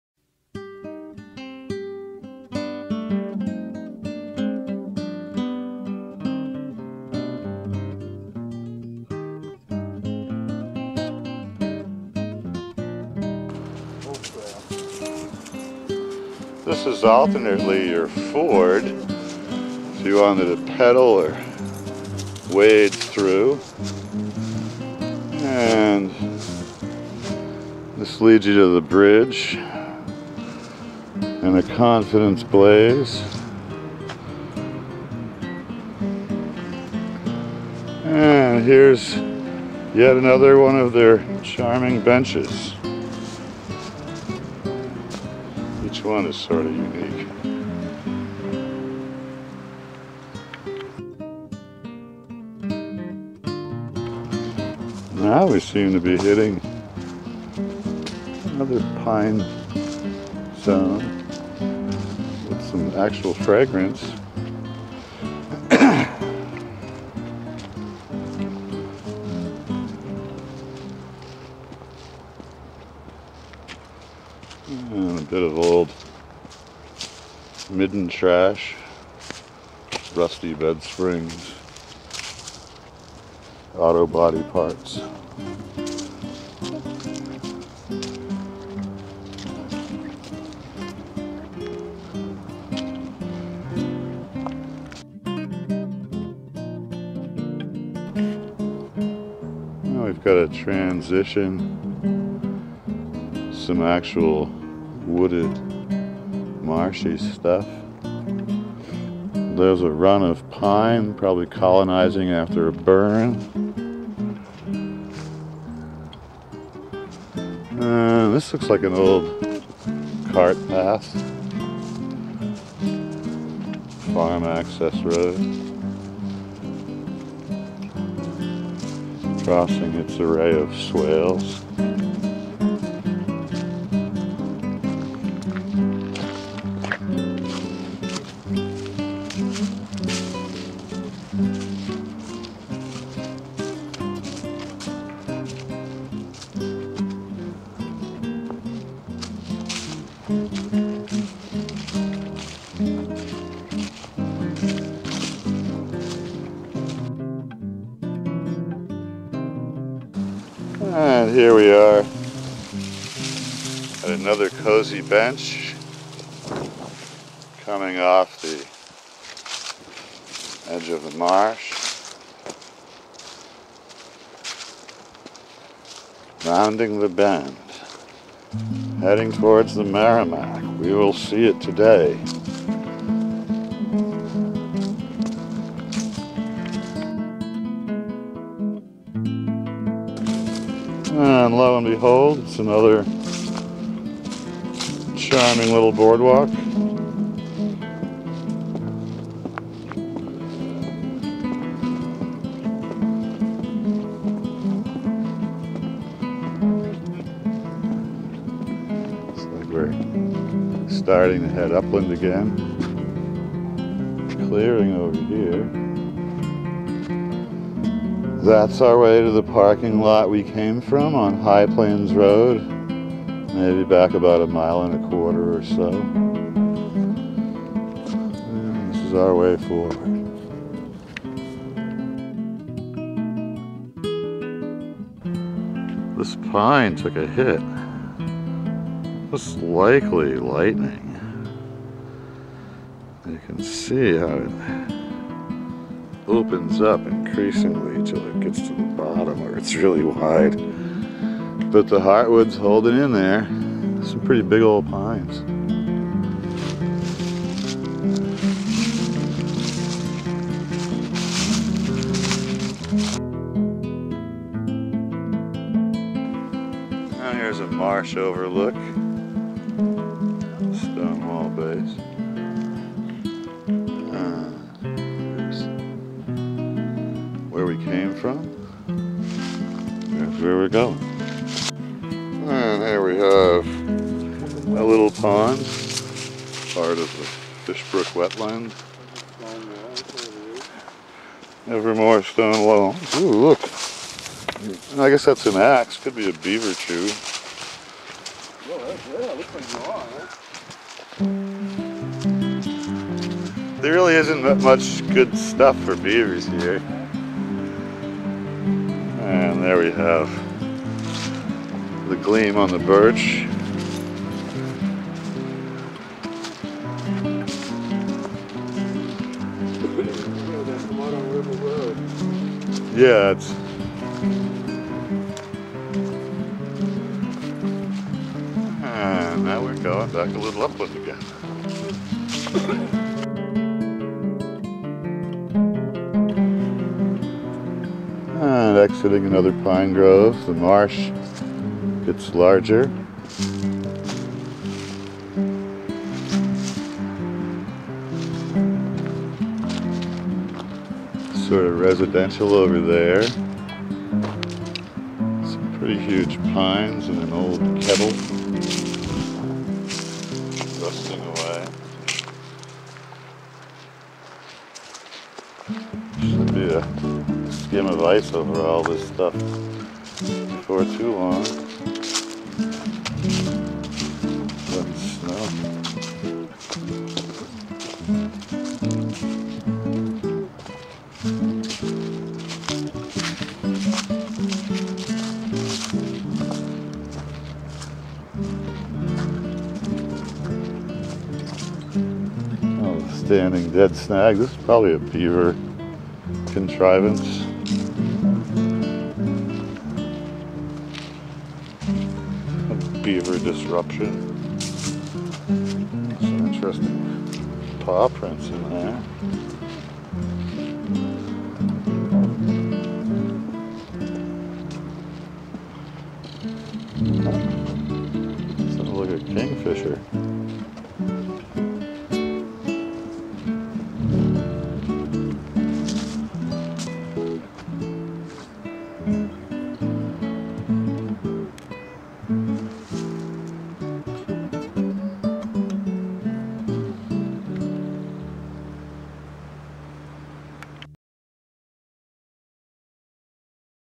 Bay Circuit Trail Andover MA: Harold R. Rafton A.V.I.S. Reservation Part 3.
This segment crosses through wetland areas and passes a number of comfy wooden benches.